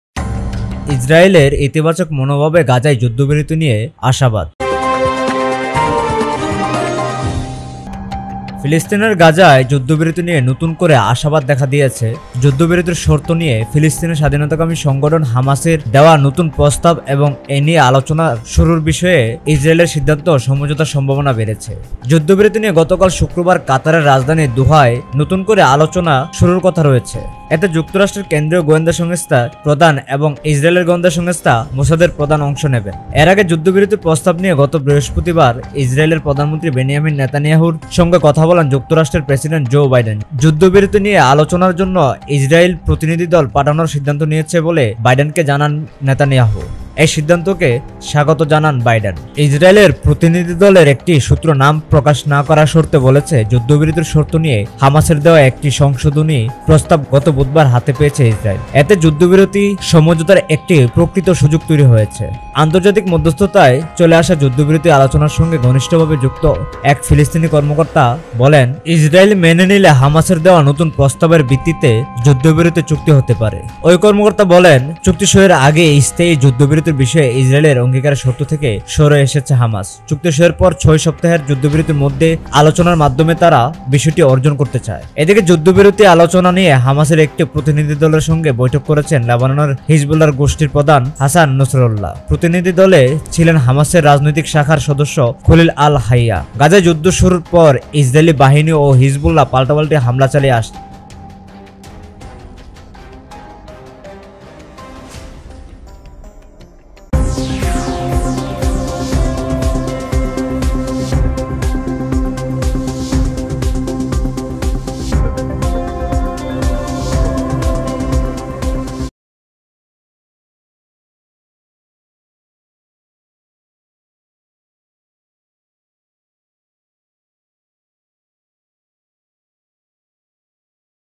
ইসরায়েলের ইতিবাচক মনোভাবে গাজায় যুদ্ধবিরতি নিয়ে আশাবাদ ।Optimism over Gaza ceasefire on positive Vtv
ফিলিস্তিনের গাজায় যুদ্ধবিরতি নিয়ে নতুন করে আশাবাদ দেখা দিয়েছে। যুদ্ধবিরতির শর্ত নিয়ে ফিলিস্তিনের স্বাধীনতাকামী সংগঠন হামাসের দেওয়া নতুন প্রস্তাব এবং এ নিয়ে আলোচনা শুরুর বিষয়ে ইসরায়েলের সিদ্ধান্তে সমঝোতার সম্ভাবনা বেড়েছে।

যুদ্ধবিরতি নিয়ে গতকাল শুক্রবার কাতারের রাজধানী দোহায় নতুন করে আলোচনা শুরুর কথা রয়েছে। এতে যুক্তরাষ্ট্রের কেন্দ্রীয় গোয়েন্দা সংস্থার (সিআইএ) প্রধান এবং ইসরায়েলের গোয়েন্দা সংস্থা মোসাদের প্রধান অংশ নেবেন।

এর আগে যুদ্ধবিরতি প্রস্তাব নিয়ে গত বৃহস্পতিবার ইসরায়েলের প্রধানমন্ত্রী বেনিয়ামিন নেতানিয়াহুর সঙ্গে কথা বলেন যুক্তরাষ্ট্রের প্রেসিডেন্ট জো বাইডেন। যুদ্ধবিরতি নিয়ে আলোচনার জন্য ইসরায়েল প্রতিনিধিদল পাঠানোর সিদ্ধান্ত নিয়েছে বলে বাইডেনকে জানান নেতানিয়াহু। এ সিদ্ধান্তকে স্বাগত জানান বাইডেন।
ইসরায়েলের প্রতিনিধিদলের একটি সূত্র নাম প্রকাশ না করার শর্তে বলেছে, যুদ্ধবিরতির শর্ত নিয়ে হামাসের দেওয়া একটি সংশোধিত প্রস্তাব গত বুধবার হাতে পেয়েছে ইসরায়েল। এতে যুদ্ধবিরতি সমঝোতার একটি প্রকৃত সুযোগ তৈরি হয়েছে।

আন্তর্জাতিক মধ্যস্থতায় চলে আসা যুদ্ধবিরতি আলোচনার সঙ্গে ঘনিষ্ঠভাবে যুক্ত এক ফিলিস্তিনি কর্মকর্তা বলেন, ইসরায়েল মেনে নিলে হামাসের দেওয়া নতুন প্রস্তাবের ভিত্তিতে যুদ্ধবিরতি চুক্তি হতে পারে।
ওই কর্মকর্তা বলেন, চুক্তি সইয়ের আগেই স্থায়ী যুদ্ধবিরতির বিষয়ে ইসরায়েলের অঙ্গীকারের শর্ত থেকে সরে এসেছে হামাস। চুক্তি সইয়ের পর ছয় সপ্তাহের যুদ্ধবিরতির মধ্যে আলোচনার মাধ্যমে তারা বিষয়টি অর্জন করতে চায়।

এদিকে যুদ্ধবিরতি আলোচনা নিয়ে হামাসের একটি প্রতিনিধিদলের সঙ্গে বৈঠক করেছেন লেবাননের হিজবুল্লাহ গোষ্ঠীর প্রধান হাসান নসরুল্লাহ। প্রতিনিধিদলে ছিলেন হামাসের রাজনৈতিক শাখার সদস্য খলিল আল-হাইয়া। গাজায় যুদ্ধ শুরুর পর ইসরায়েলি বাহিনী ও হিজবুল্লাহ পাল্টাপাল্টি হামলা চালিয়ে আসছে।